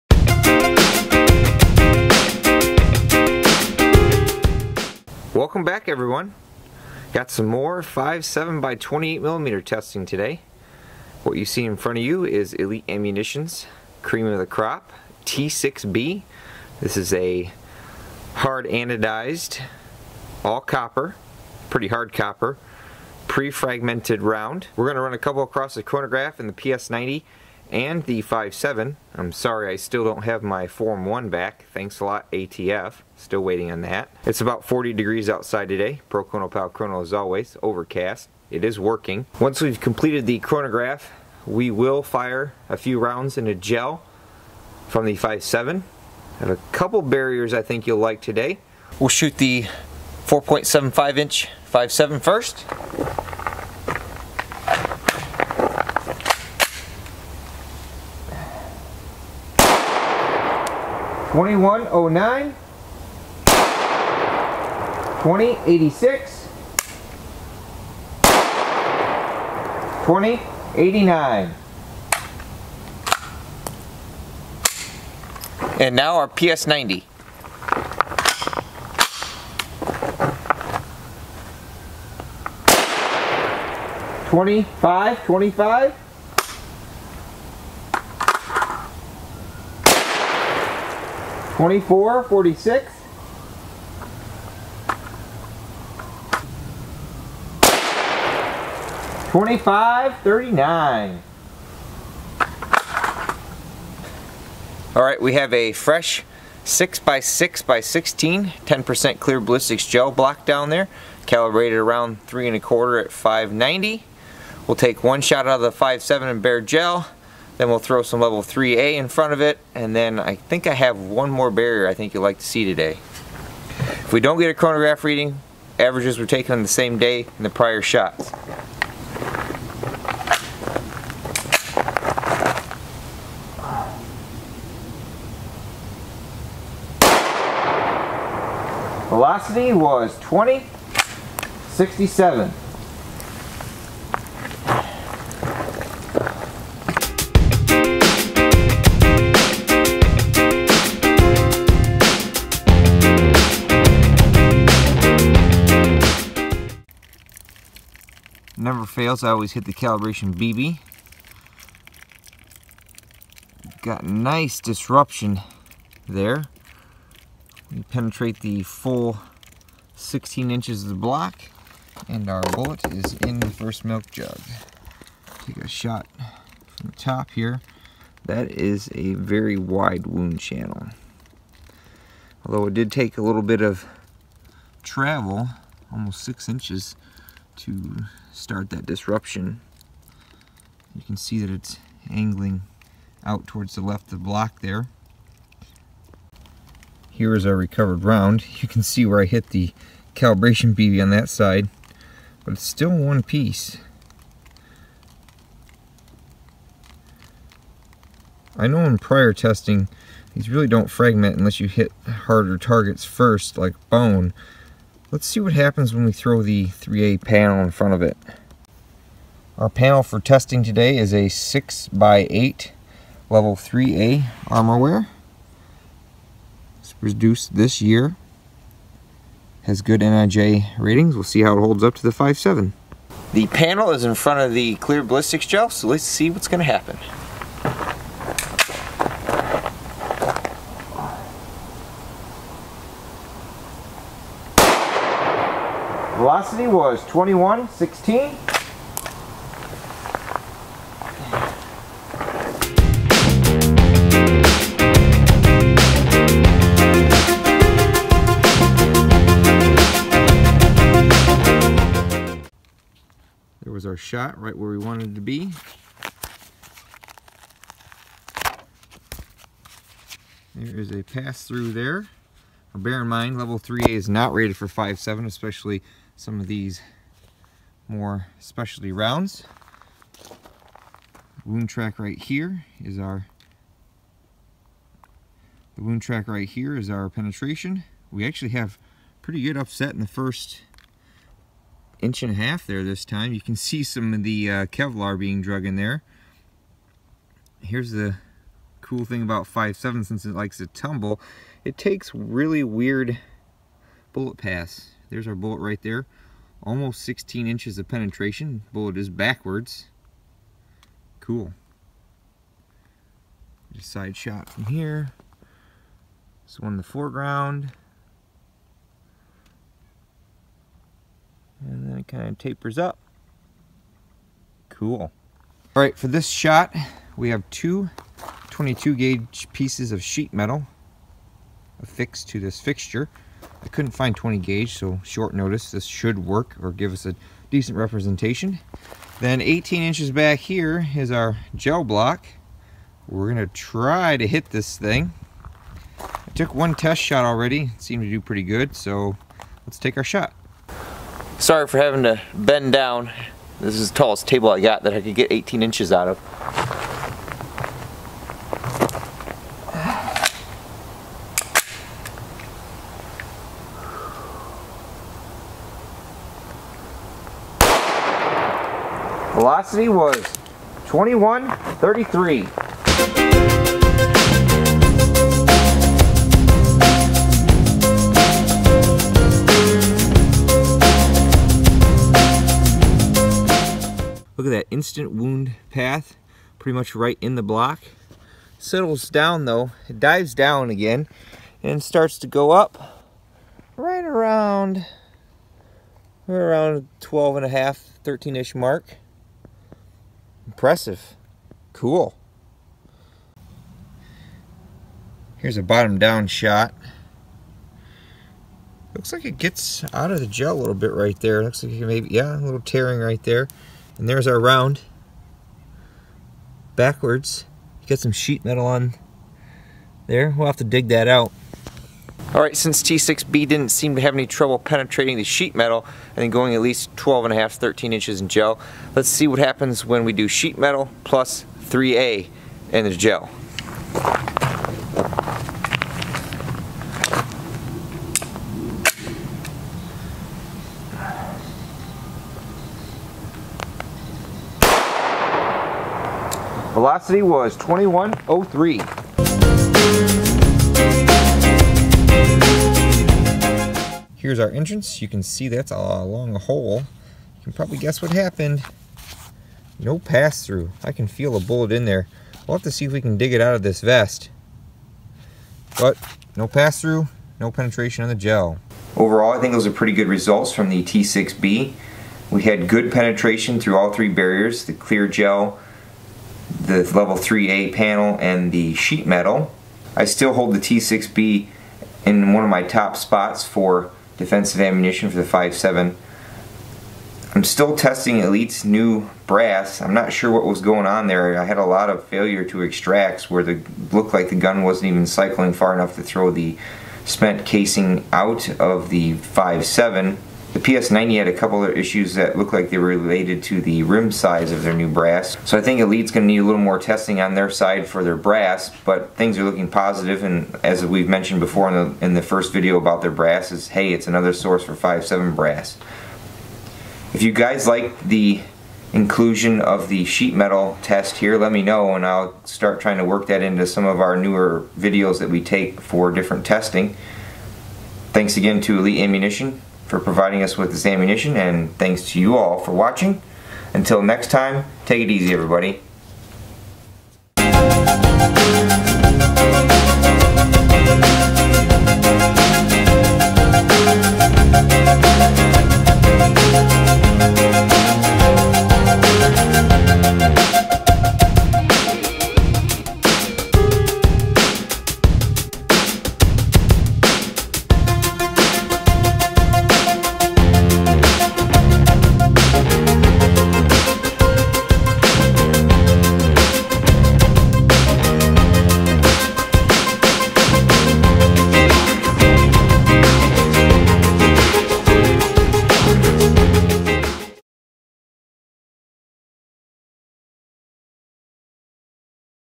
5.7x28mm, 32gr PFP, T6B Elite Ammunition, Ballistic Review
3/20
Gel Used: 10% Clear Ballistic Gel:

Weather:
Temperature: 45°F
Humidity: 85%
Baro: 30.23"HG
Dew Point: 40.60°F
Elevation: 849 Feet.

All shots are in feet per second (fps)

Five-seveN:
1. 2,109
2. 2,086
3. 2,089
Avg: 2,095
312 ft lbs.

PS90:
1. 2,525
2. 2,446
3. 2,539
Avg: 2,503
445 ft lbs.

Gel:
Shot 1 impacted at 2,067 fps.  Full 16" of penetration plug into 1 milk jug. PFP points did not fragment. Very wide wound channel. Neck was fairly long (6").

Level IIIA:
Shot 1 impacted at 2,116 fps. Penetrated the vest, and almost 16" of penetration present. A lot shorter neck and still fairly large wound channel.

Sheet Metal:
Shot 1 impacted at 2,133 fps. Bullet started to yaw because of the sheet metal. 12.5-13" of penetration present, and decent wound cavity.  Some sheet metal was carried with the bullet..

Sheet Metal With Armor:
Shot 1 impacted at 2,103 fps. Bullet failed to penetrate the armor panel, as it was yawing upon impact..

This is one of my top 5.7 picks..